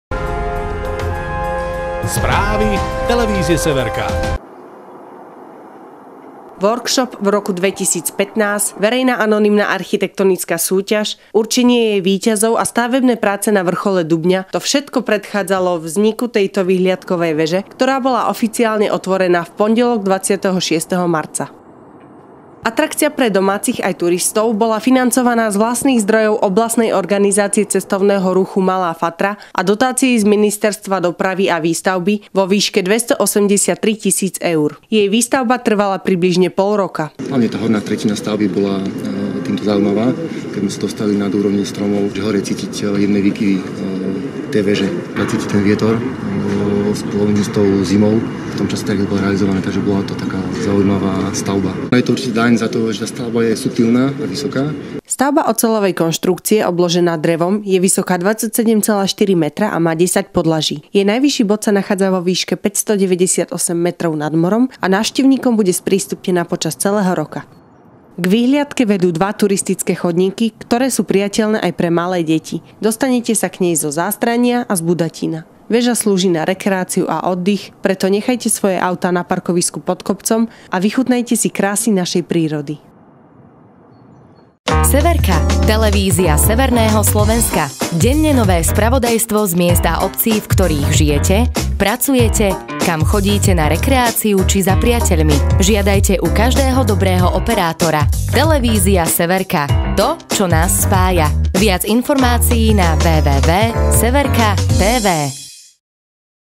TV SEVERKA Vyhliadková veža na Dubni
Celoročná atrakcia pre domácich aj turistov.

ŽILINA. Na Dubni bola vybudovaná nová vyhliadková veža vysoká takmer 30 metrov.